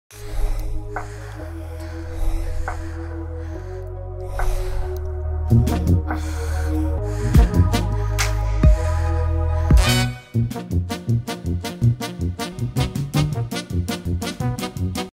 Okamoto Condoms: Grandmother - Marcus Thomas
Okamoto: Grandmother
Ads Via Marcus Thomas Take the Fun Out of Sex
Credits
Agency: Marcus Thomas Cleveland
Client: Okamoto
Executive Creative Director: Joanne Kim
Creative Director: Jim Sollisch
ACD/Art Director: Eric Holman
Copywriter: Kevin Delsanter
Digital Illustrator: Laurie Znidarsic
Stock Footage: Getty Images
Stock Footage: iStock